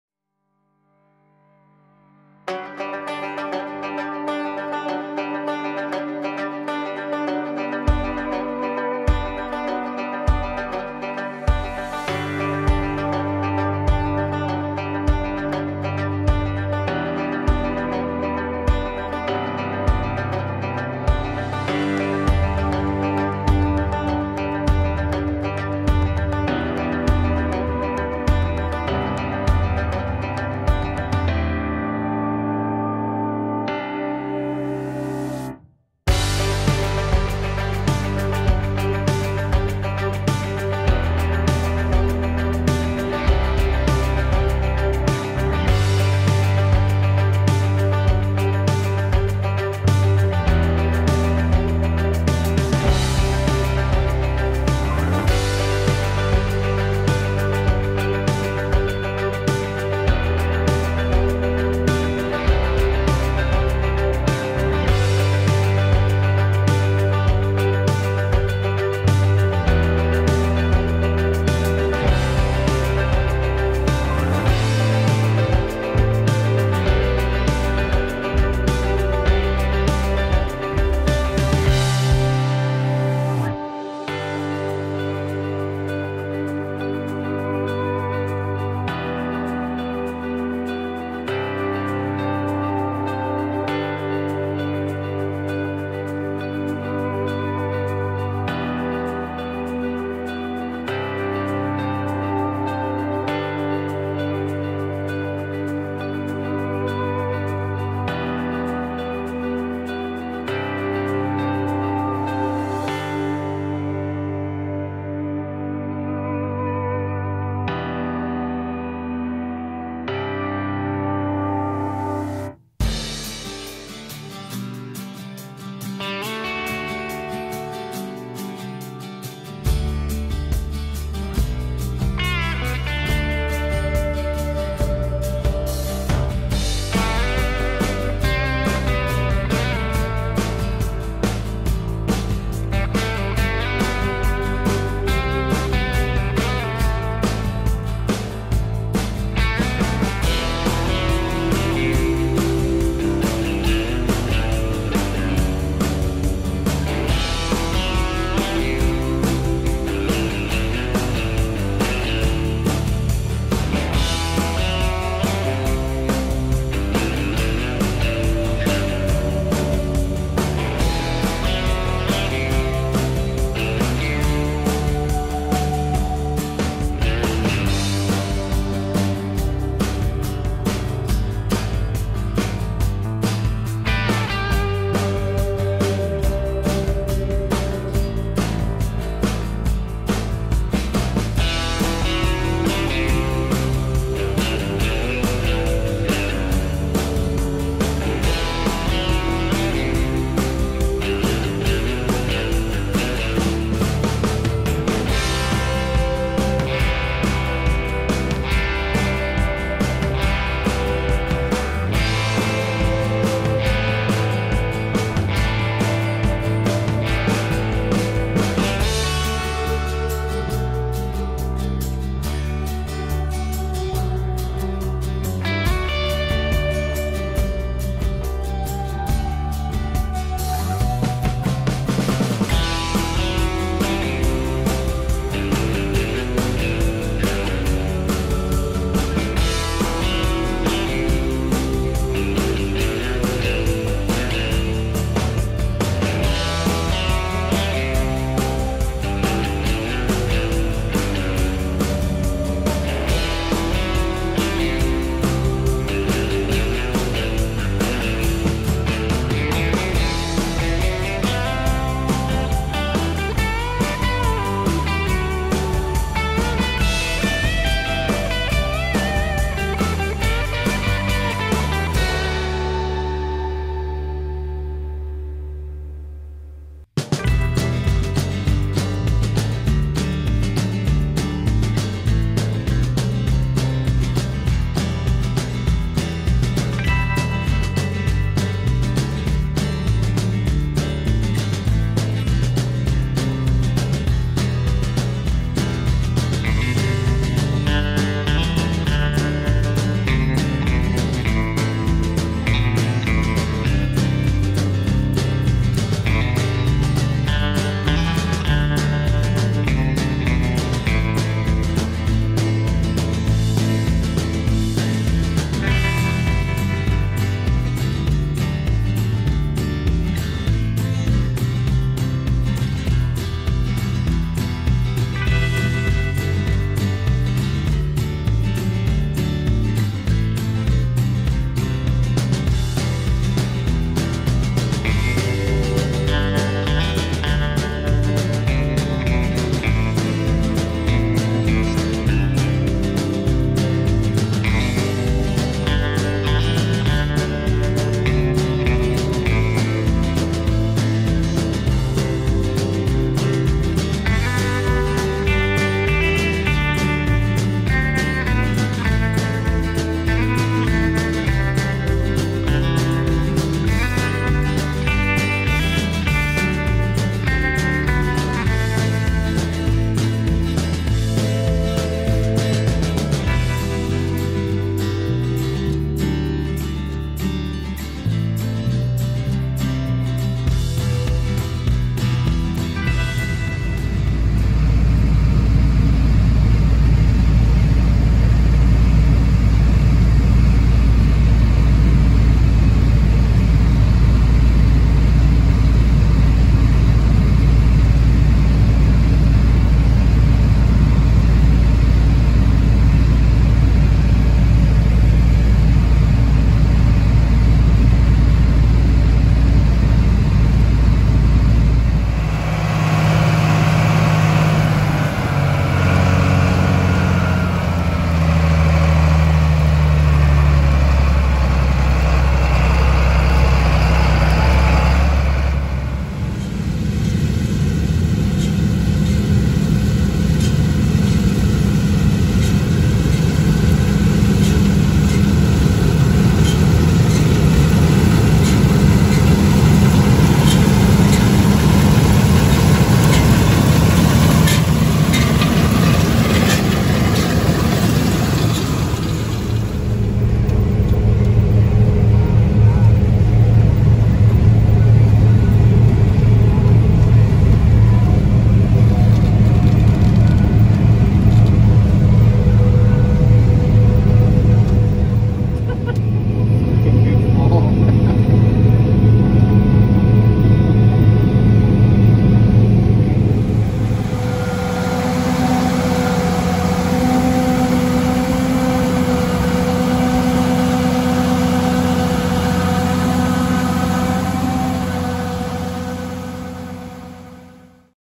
Harvest 2023 with a John Deere 9660 STS
Harvesting corn and soybeans 2023 with Thompson Farms.